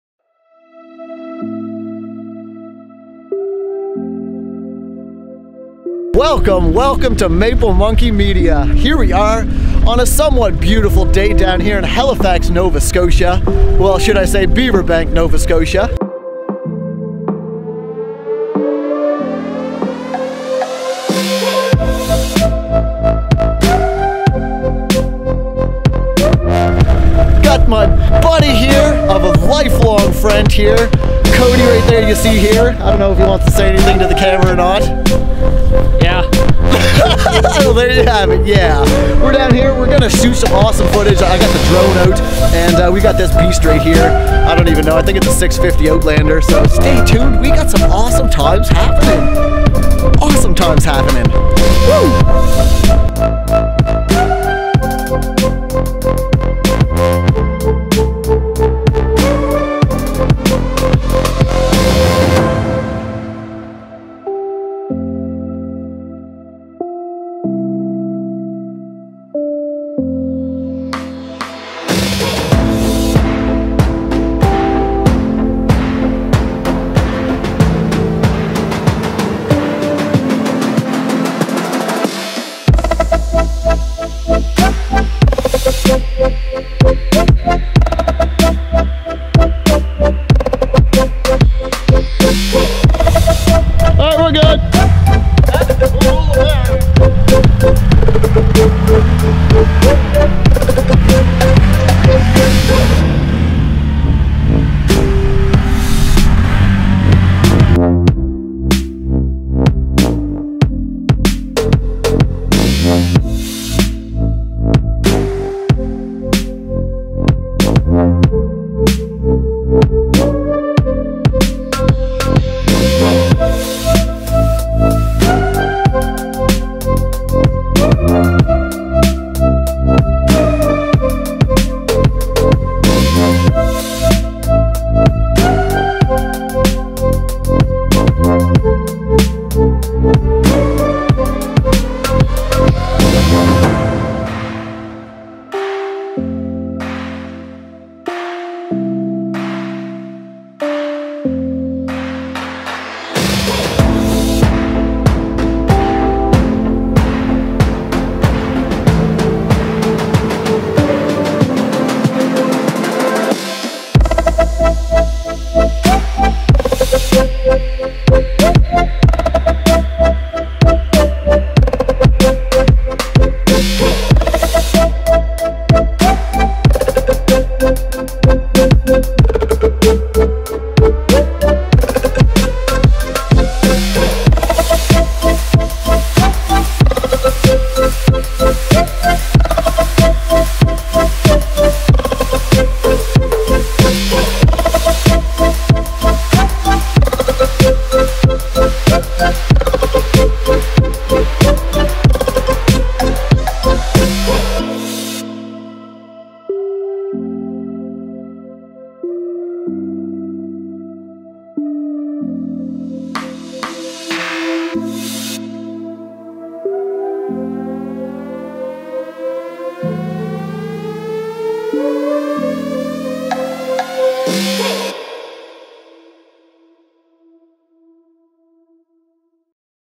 650 Can Am Outlander + DJI Mavic Air + Abandonded Military Base = EPIC FUN 2020
650 Can Am Outlander + DJI Mavic Air + Abandonded Military Base = EPIC FUN 2020

I decided I needed to upgrade and get into a whole new category of videos by purchasing the Mavic Air and I am super happy with this upgrade. I used to have the dji spark and i tell you this is definitely an upgrade from that.
I wanted to go try it out and see what it was capable of and thats exactly what I did, i called up my friend to see if he wanted to come along and bring his bike for a little fun at the abandoned military radar base that is in the area i live in. It was an amazing time checking out the abandoned base and flying the dji mavic air over head. I am slowly getting better at flying and maneuvering it and there are going to be plenty more drone videos coming up so stay tuned and hit that subscirbe button to stay up to date